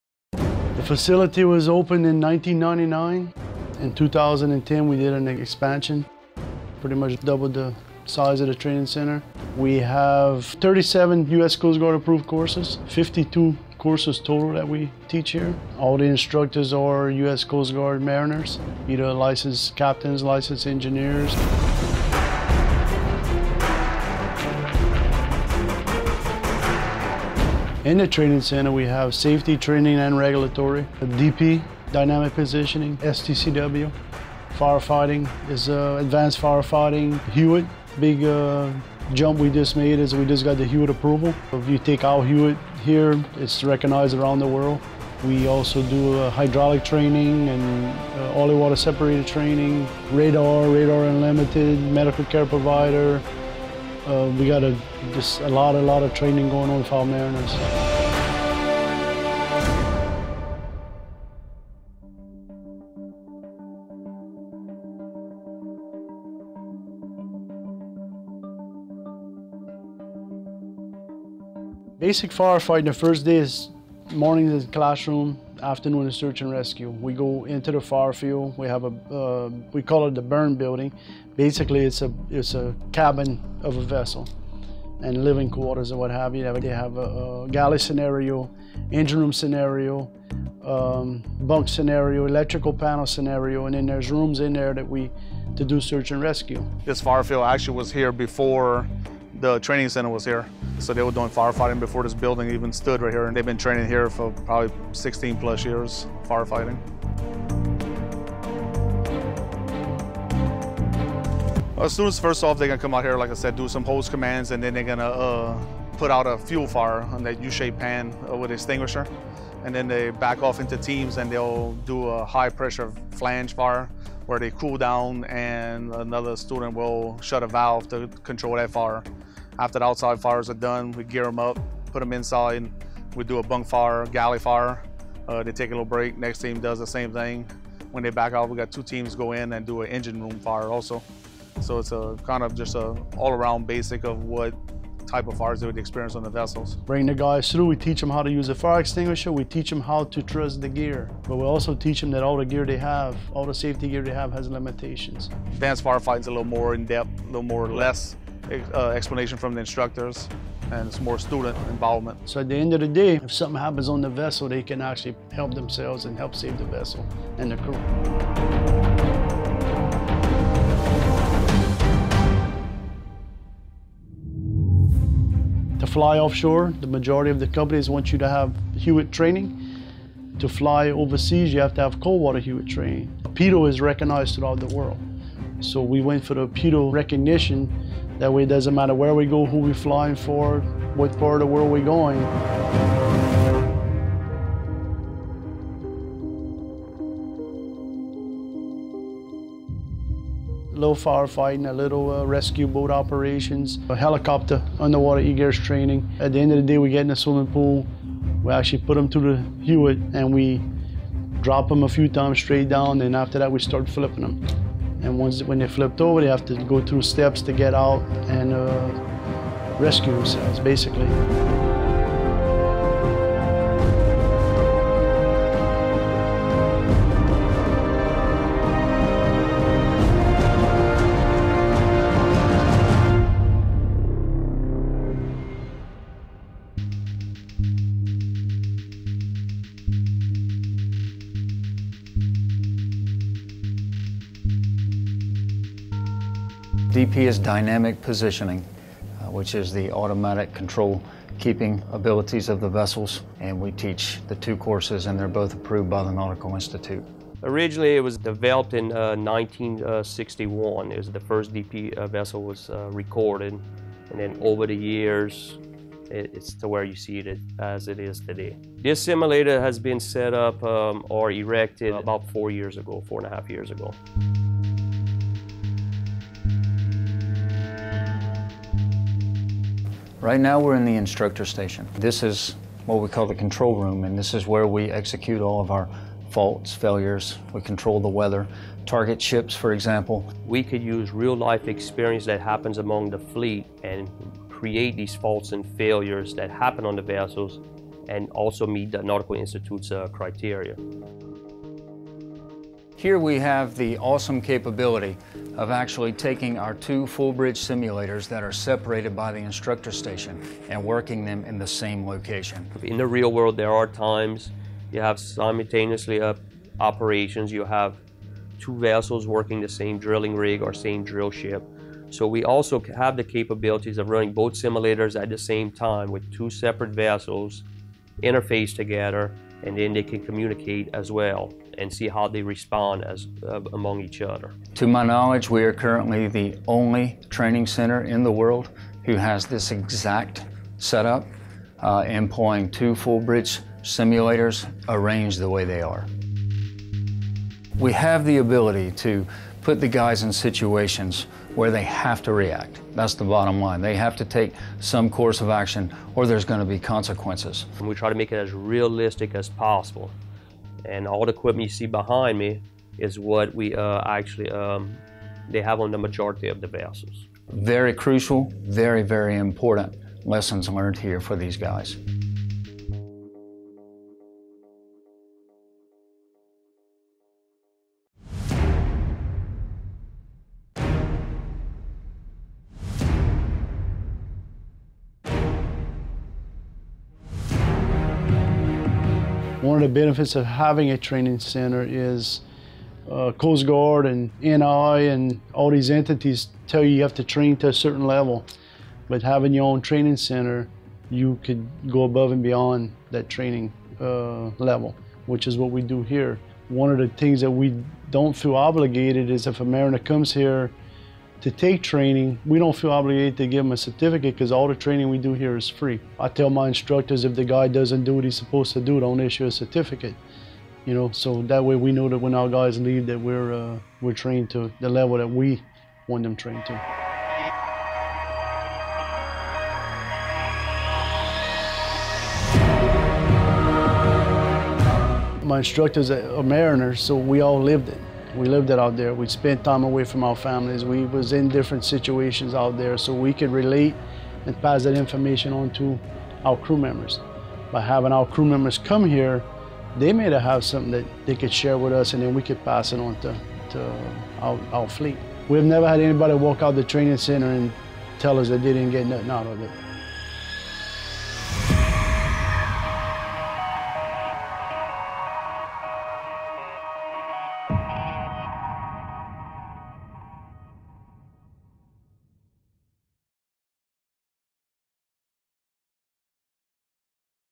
ECO Training Center
ECO's state-of-the-art training center is one of the most advanced in the world. Staffed by Coast Guard-certified instructors, the ECO Training Center offers a wide range of classroom instruction, as well as simulated exercises in water survival, firefighting and advanced vessel control.